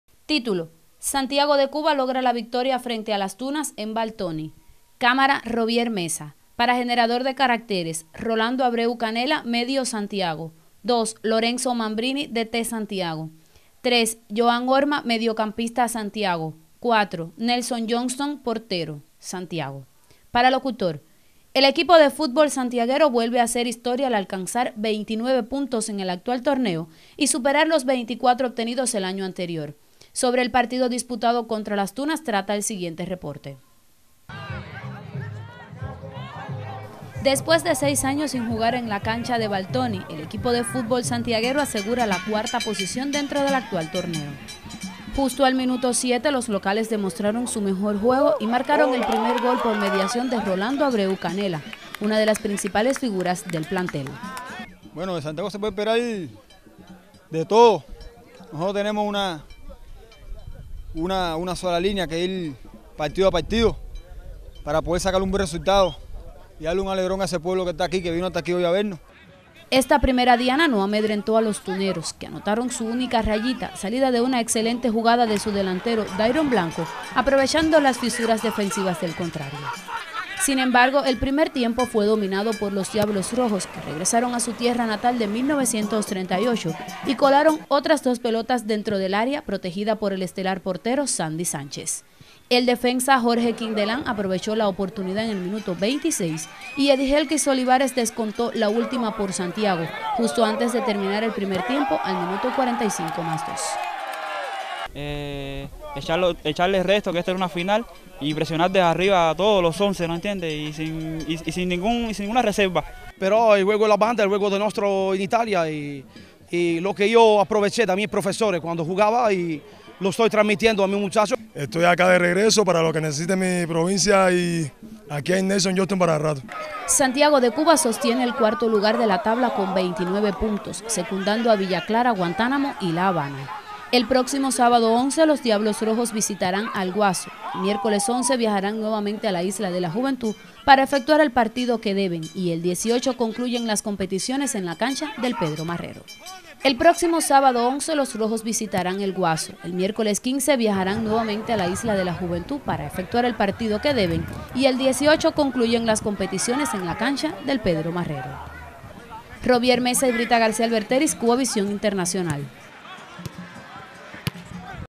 Reportaje Futbol CubaVisión Internacional
el equipo santiaguero de futbol esta a unos pasos de incluirse dentro del medallero del Campeonato 101.  Lorenzo Mambrini con un gran trabajo.